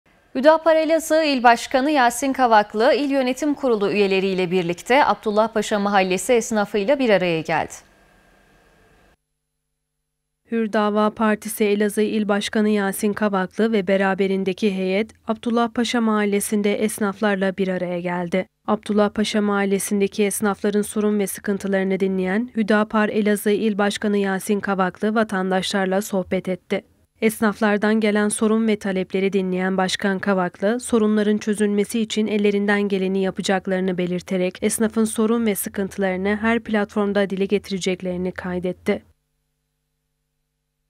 BAŞKAN KAVAKLI; ABDULLAHPAŞA ESNAFI İLE BİR ARAYA GELDİ
HÜDAPAR ELAZIĞ İL BŞK. YASİN KAVAKLI VE YÖNETİM KURULU ABDULLAHPAŞA MAHALLESİNDEKİ ESNAFLAR İLE BİR ARAYA GELEREK TALEPLERİ DİNLEDİ